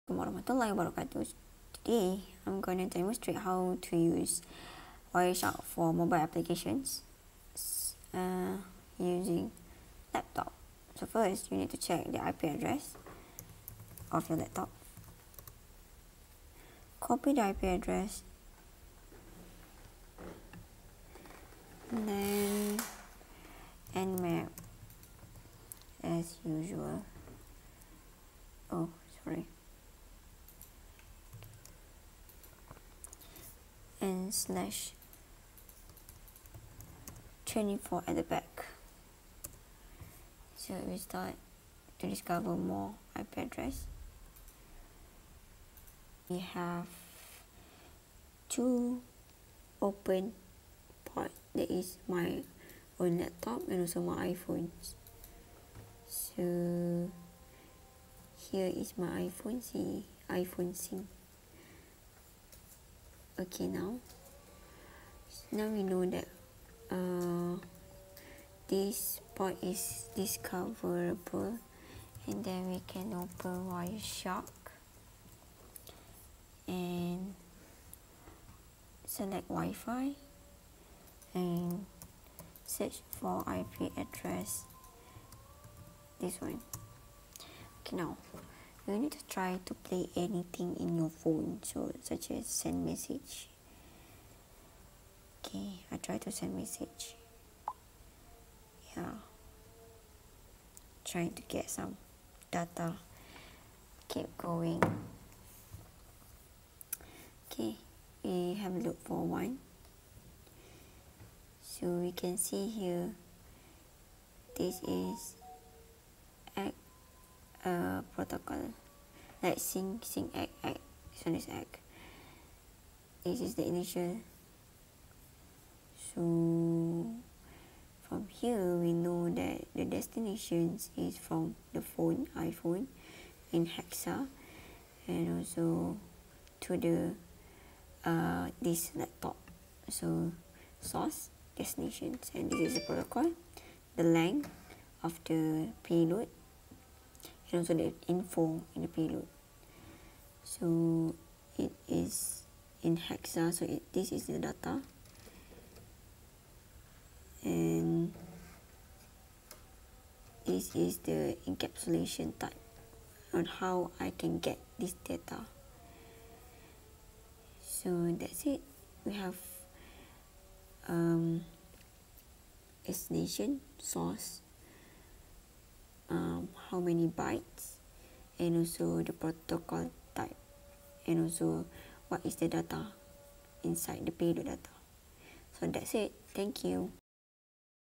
Intent Sniffer in iPhone using Wireshark and Nmap
Intent sniffer to check packet capture in mobile app.
I'm using Macbook Pro M1 chip and iPhone 8 for this tutorial.